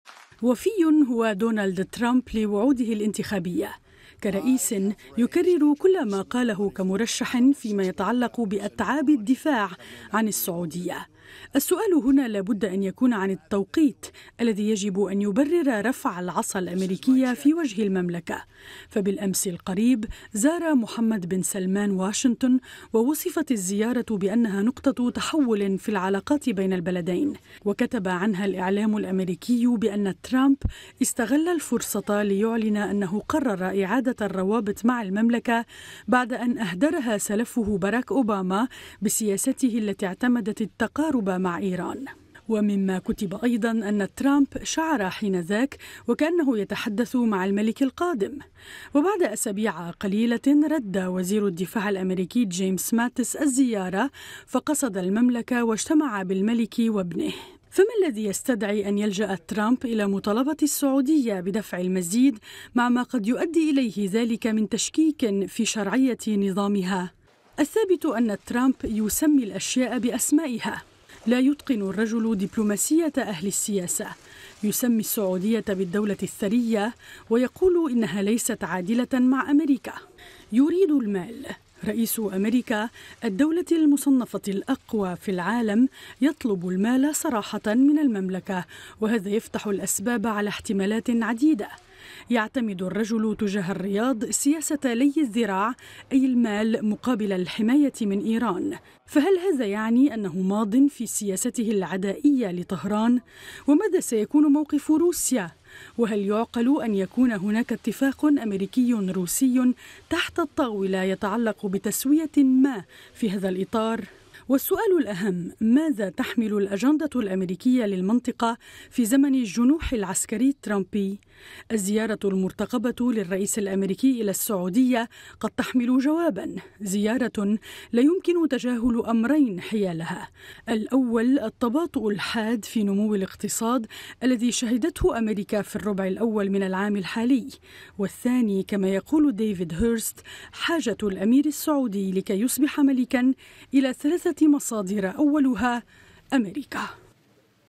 هل يطالب ترامب السعودية بدفع المزيد مقابل الحماية؟
ترامب ينتقد دفع واشنطن أموالاً طائلةً للدفاع عن السعودية
الكاتب: غراسيا بيطار
إعداد: الميادين
المال مقابل الصمود؛ هذه هي المعادلة التي يحملها الرئيس الأميركي دونالد ترامب في وجه السعودية معادلة يقال فيها الكثير.